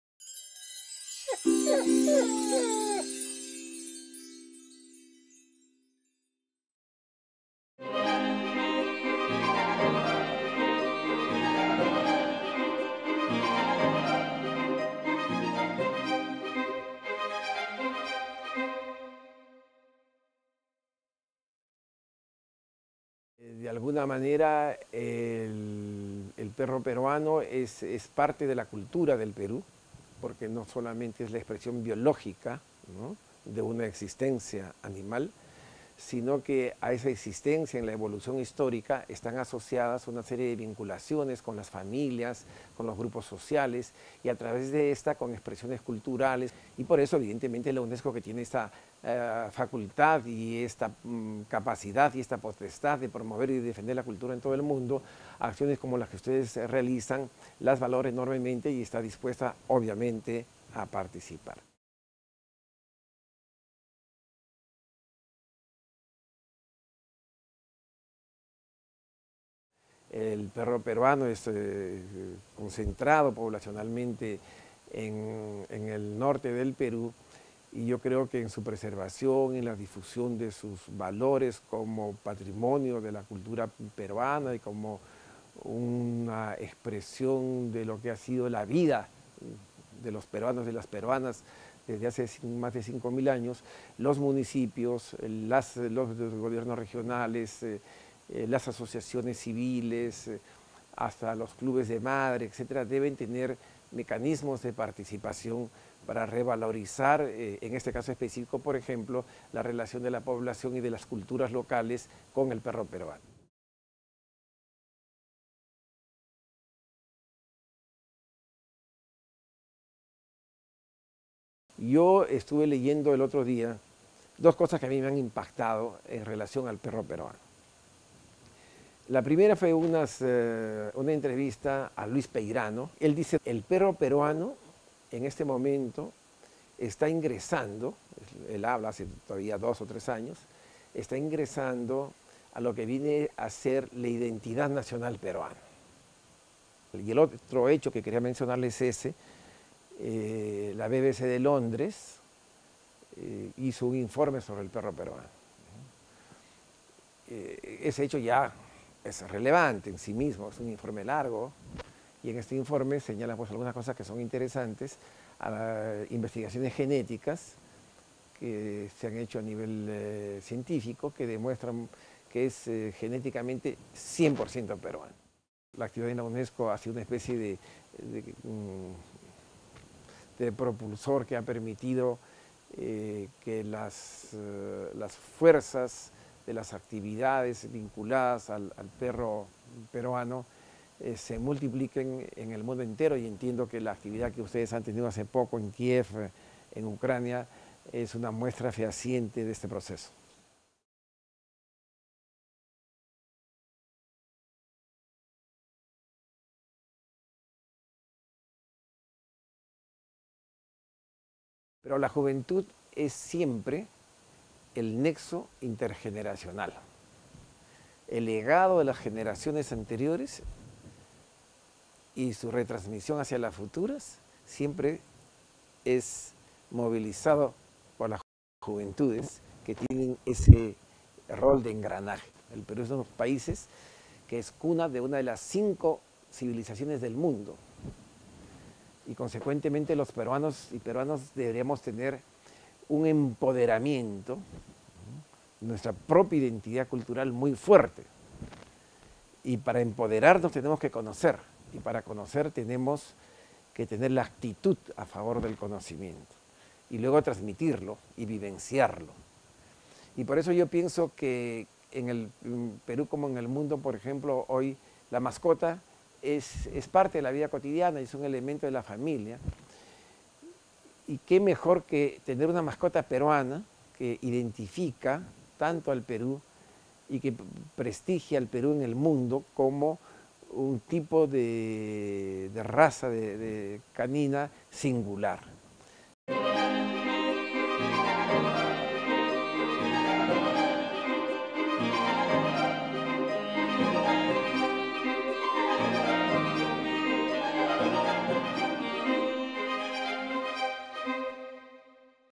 PERUÁNSKÝ NAHÁC: VZKAZ MANUELA RODRIGUEZE CUADROSE, VELVYSLANCE PERU PŘI UNESCO – VO/CZ
"PERUÁNSKÝ NAHÁC"
"Kořeny » Tour 2017"
Vzkaz Manuela Rodrígueze Cuadrose,
velvyslance Peru při UNESCO,
na ochranu Peruánského naháče!
“EL PERRO SIN PELO DEL PERÚ”
El tour “Raíces” 2017
Mensaje del Embajador de la
Representación permanente del Perú ante la UNESCO
Manuel Rodríguez Cuadros
en defensa del Perro sin Pelo del Perú!